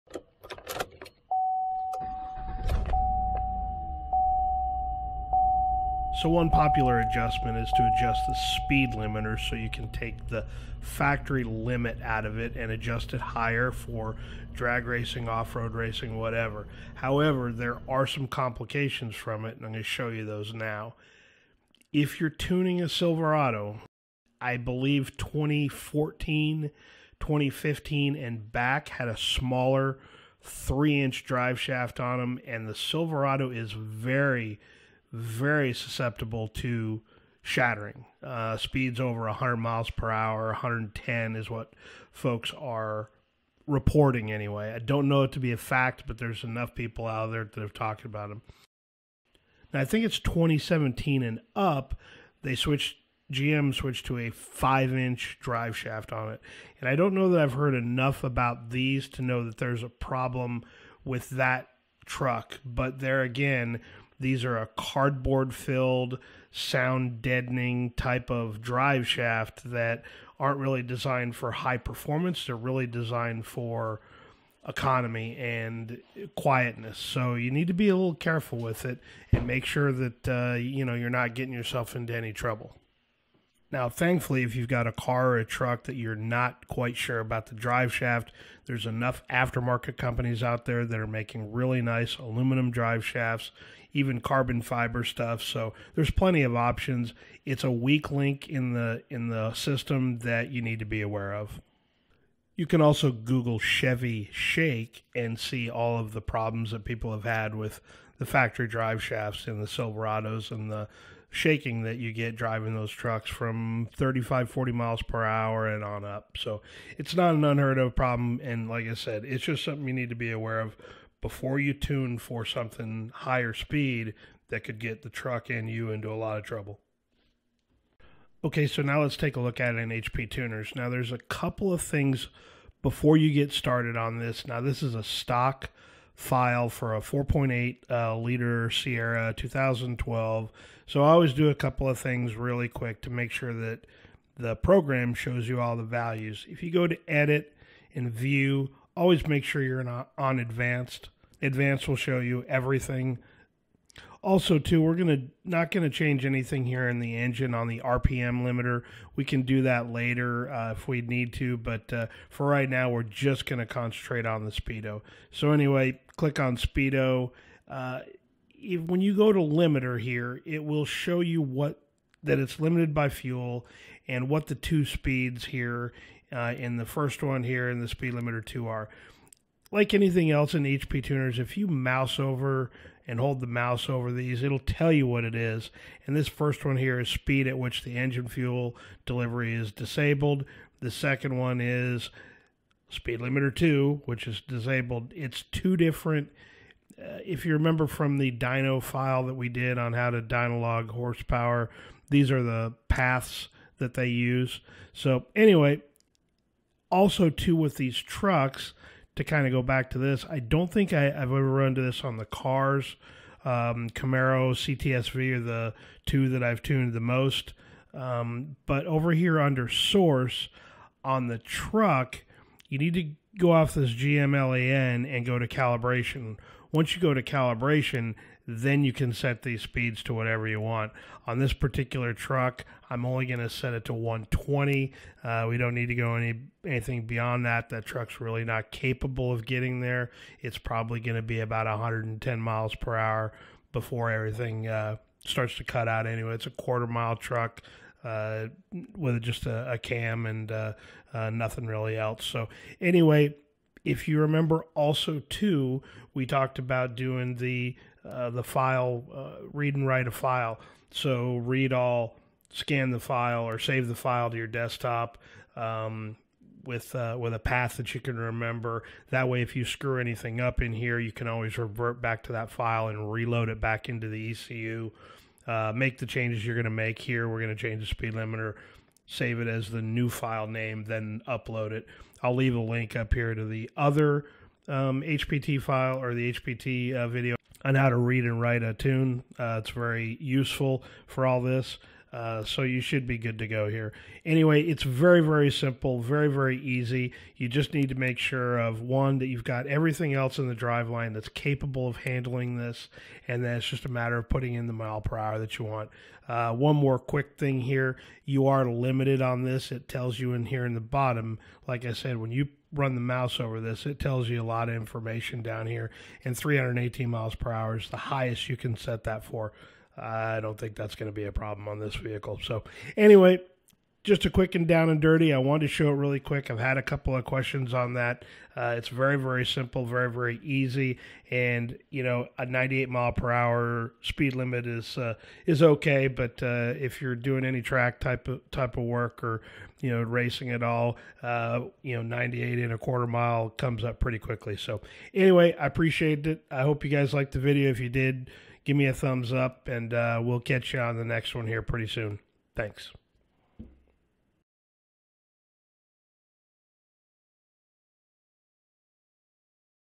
How to Raise Speed Limiter with HP Tuners
Changing the speed limiter isn't difficult, but there are some pitfalls to be aware of.  This video is for demonstration use only.  Care must be taken with factory computers.  Any modification to them can result in adverse affects.

HP Tuners MPVI2
GM Tuning School Beginners Course

New cam!

How to raise the speed limiter with HP Tuners software.  This video shows how to raise the speed limiter that all factory vehicles have to something a littler better for your track day activities.